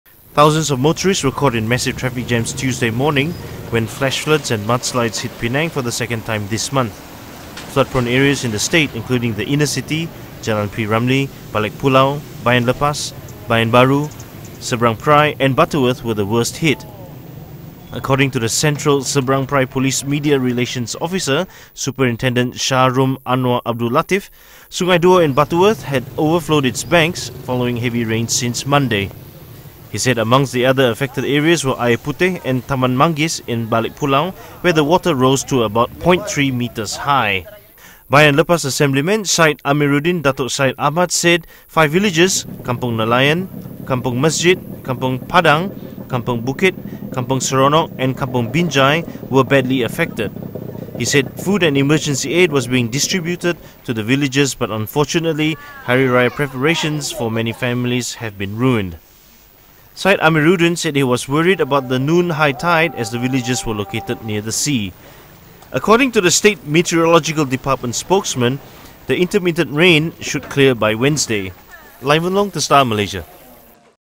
Floods, landslides hit Penang again
Flooding in Penang causes thousands of motorists caught in massive traffic jams and mudslides hit the island for the second time this month.  Hari Raya preparations for many Muslim families have been affected.